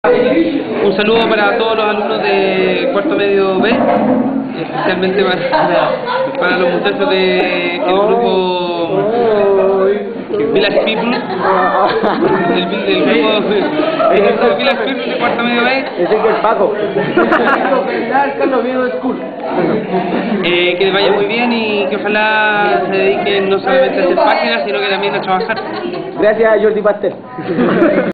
al marco hablando was
el profe marko hblando was,mientras el chiko lo webea